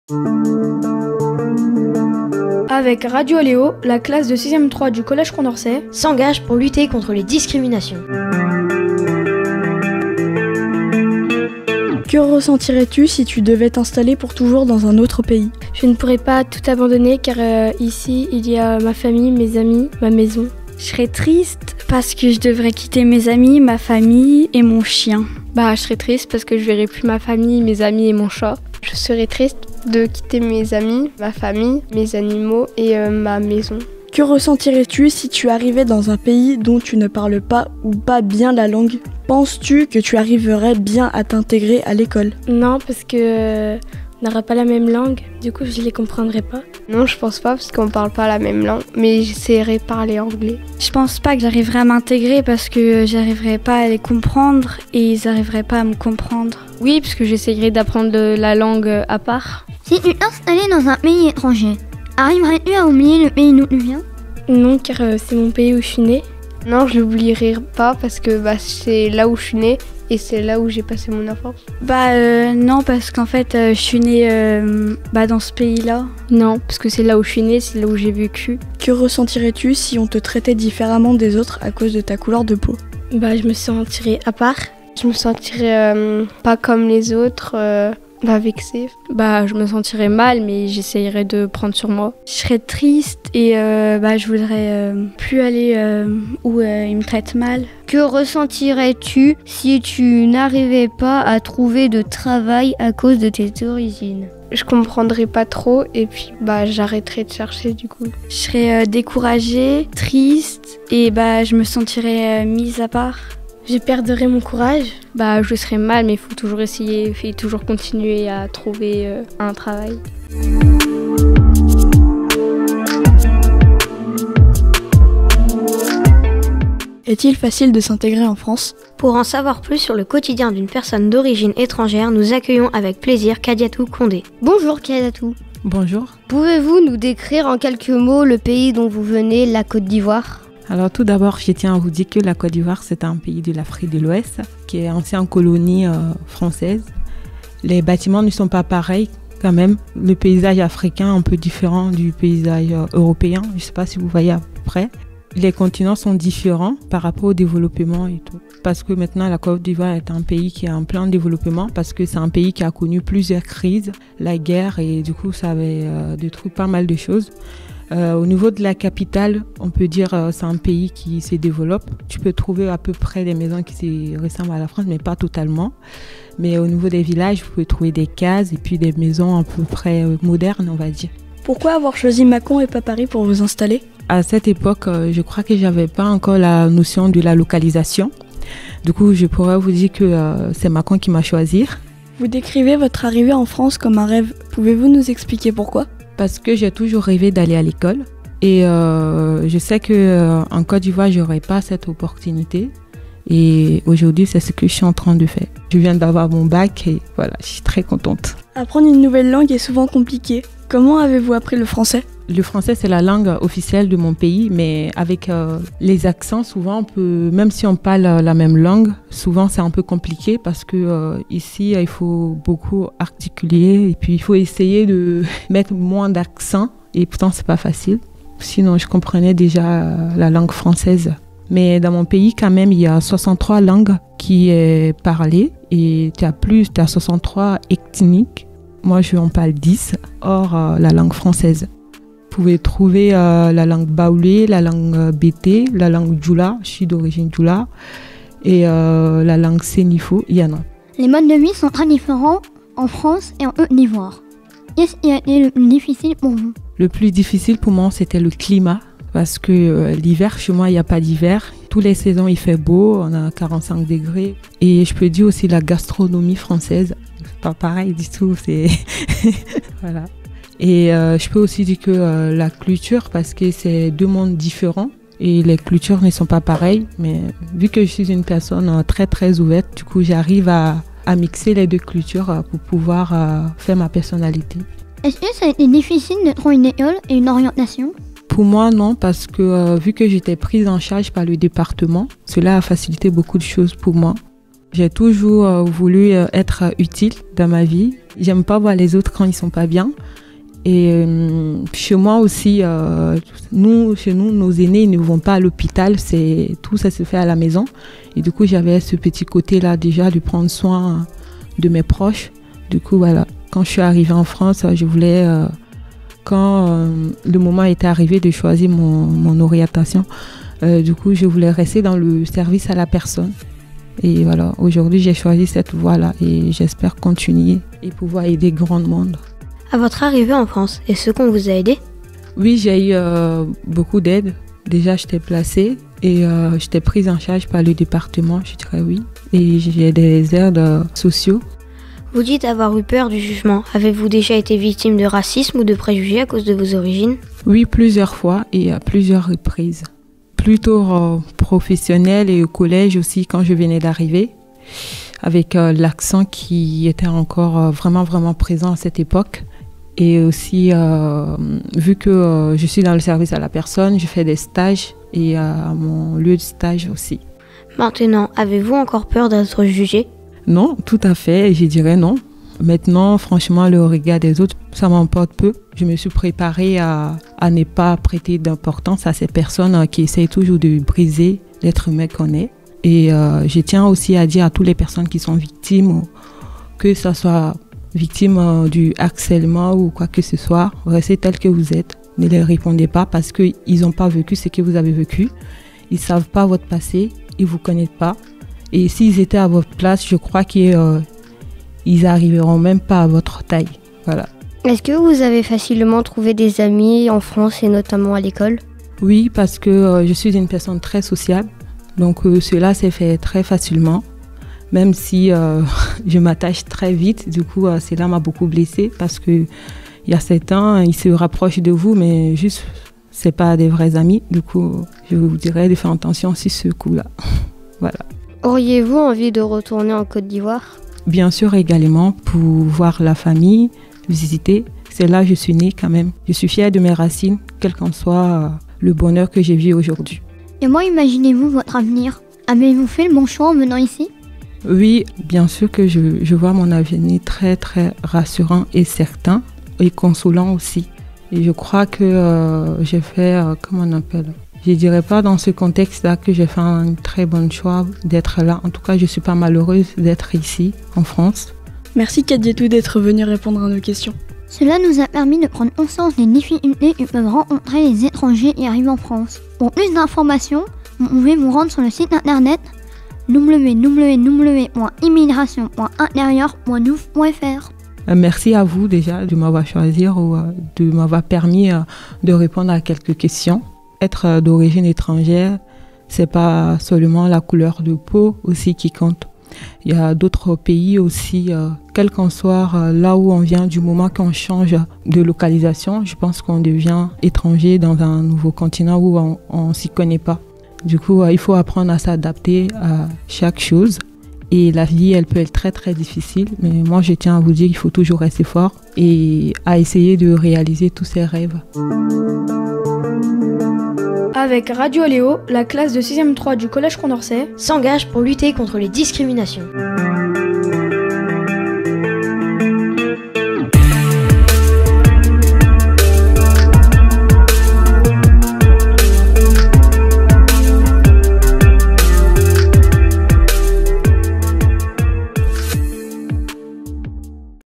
"Je suis d'origine étrangère" : Kadiatou Condé
Emission 03 - "Je suis d'origine étrangère" : Kadiatou Condé

Pour la 5e année, Sandra Thillier, journaliste de Radio Aléo, et Pascale Thibert, professeure de français au Collège Condorcet de La Chapelle de Guinchay, ont uni leurs compétences pour un atelier découverte des médias et du journalisme à destination d'une classe de 6e. Les élèves ont été amenés à s'interroger, réfléchir et échanger sur le thème des discriminations avant de réaliser 7 séquences sonores et 7 émissions radio. Cet atelier est financé par le Département 71 et le FDVA.